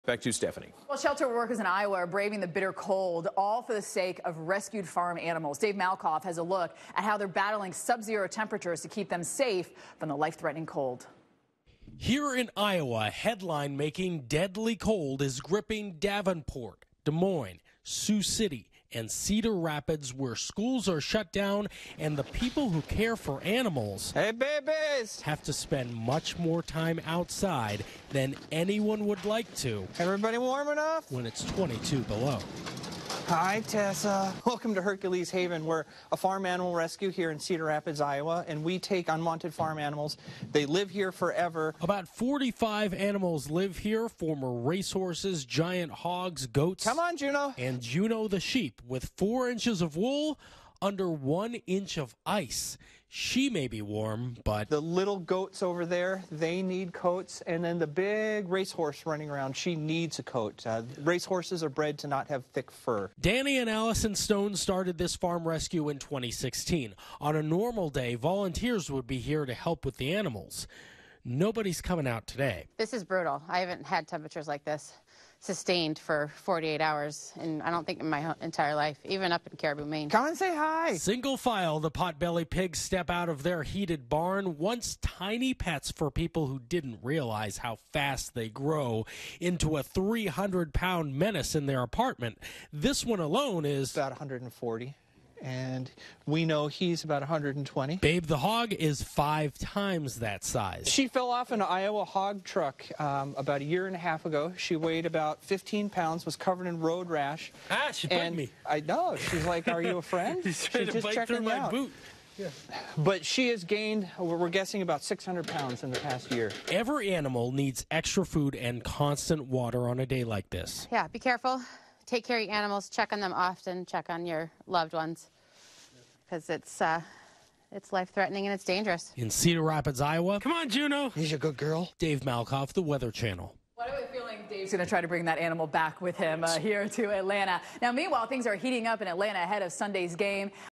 Hercules' Haven gets Barn Check help from The Weather Channel.
January 31, 2019 -- News crew from The Weather Channel visited Hercules' Haven during one of our Polar Vortex Barn Checks.